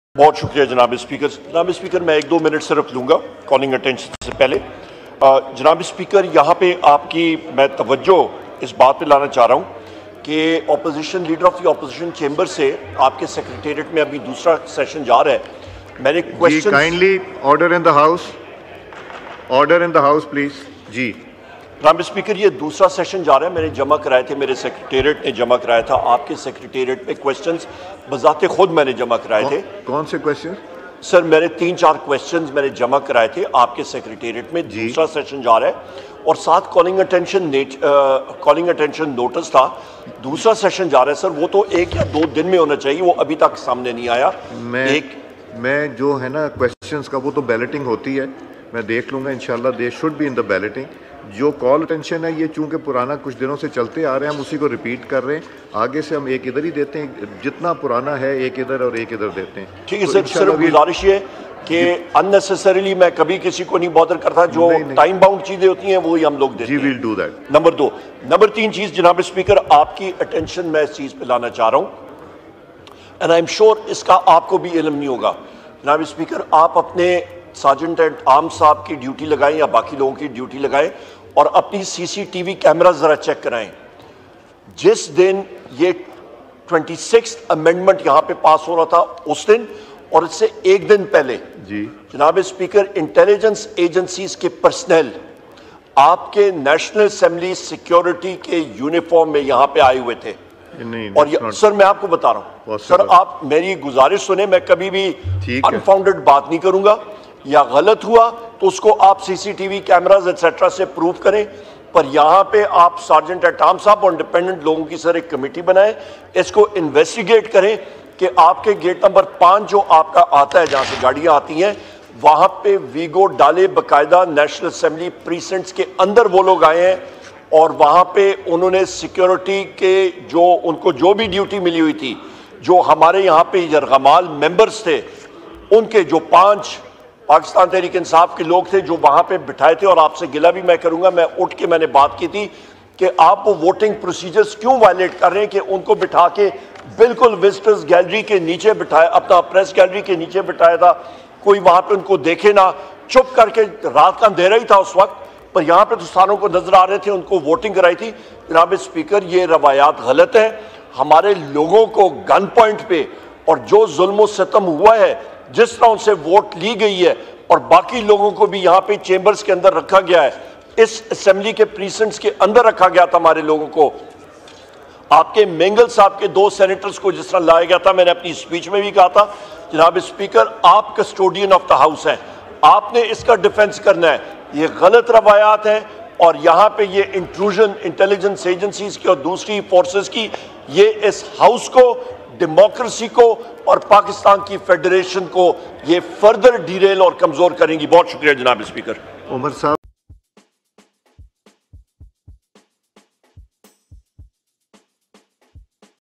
Umer Ayub Big Demand To Speakers In National Assembly, Analysis | PTI News | IK News Analysis
About Video: Umer Ayub Big Demand To Speakers In National Assembly, Analysis | PTI News | Imran Khan News Analysis